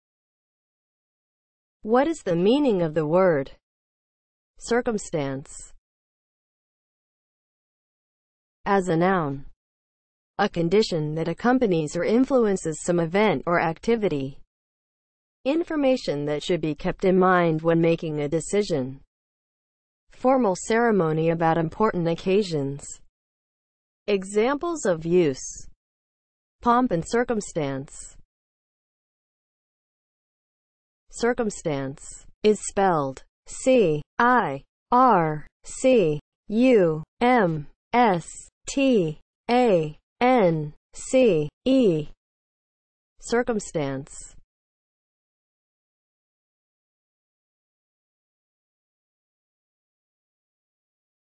What is the meaning of the word CIRCUMSTANCE?
Definition, examples of use and spelling of the word CIRCUMSTANCE.

(00:00) Intro
(00:07) As a noun
(00:29) Spelling

As a noun (00:07):
• a condition that accompanies or influences some event or activity
• information that should be kept in mind when making a decision
• formal ceremony about important occasions

Examples:
• "pomp and circumstance"